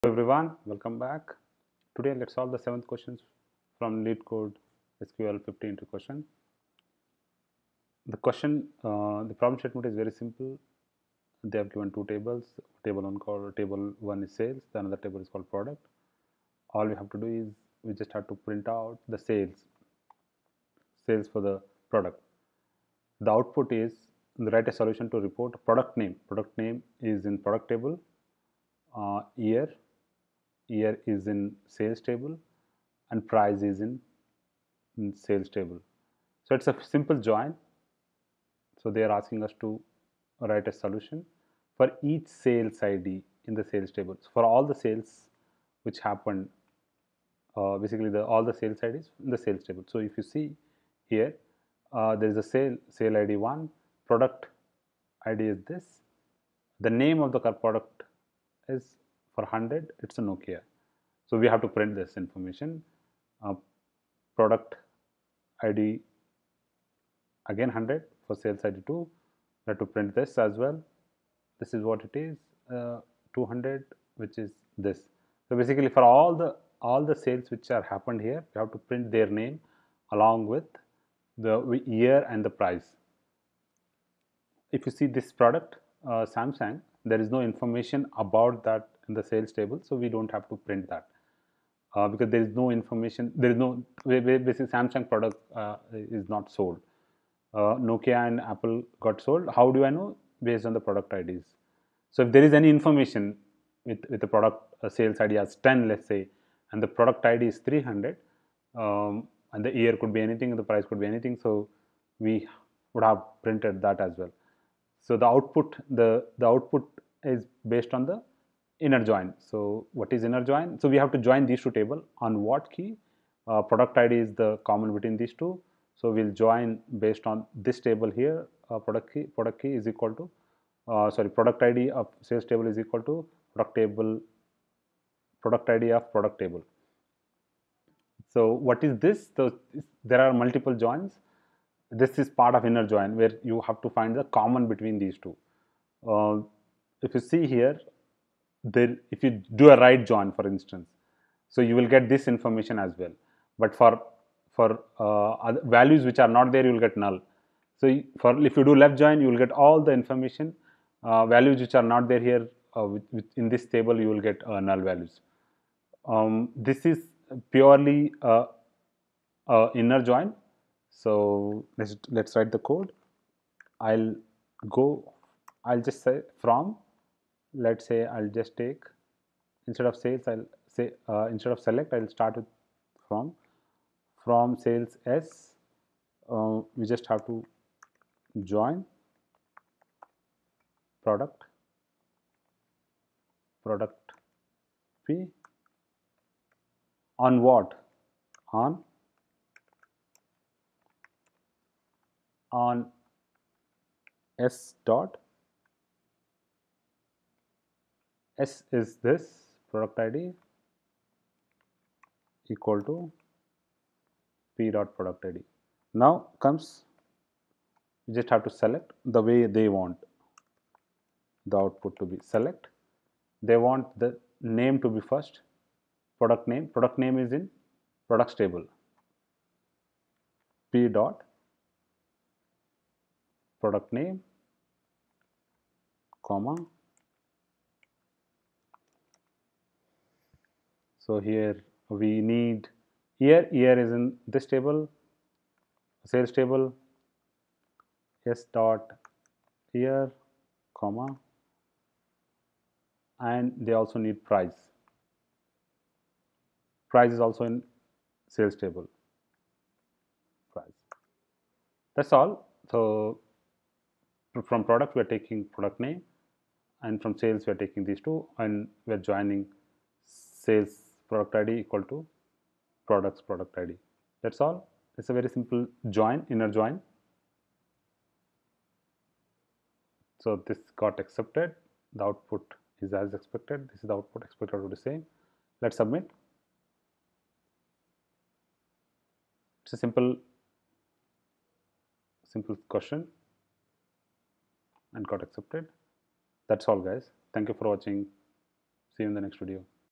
LeetcodeSQL50 - 7 Product Sales Analysis I | JOIN | SELECT | ON
Solution for Leetcode 068  Product Sales Analysis I SQL interview question.
Problem is to write a solution to report the product_name, year, and price for each sale_id in the Sales table.
By solving this problem effectively, you will learn and apply the following SQL concepts:
- JOIN
- SELECT